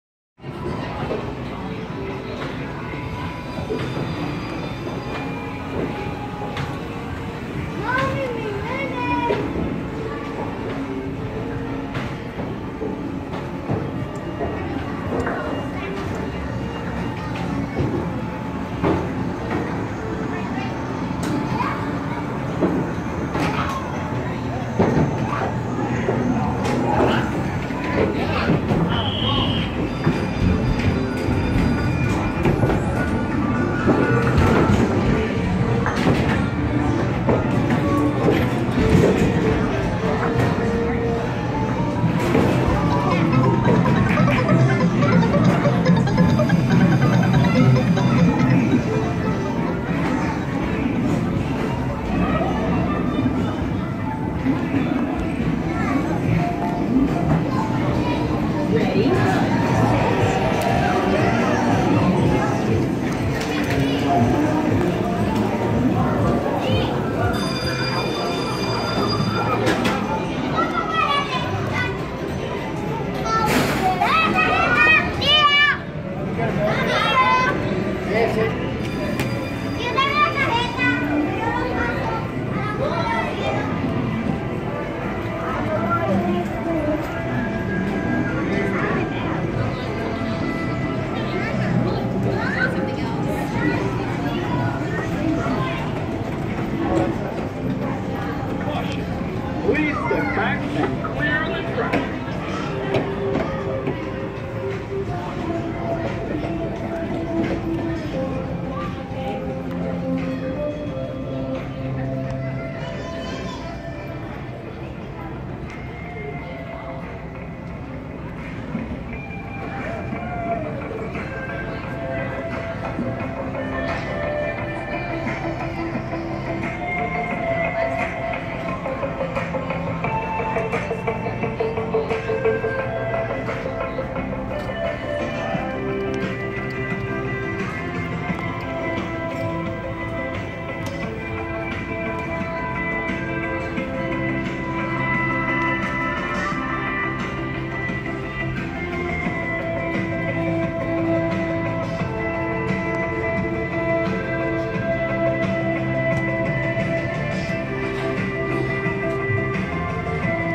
Cec store tour Brandon FL
A look around the large cec in Brandon FL.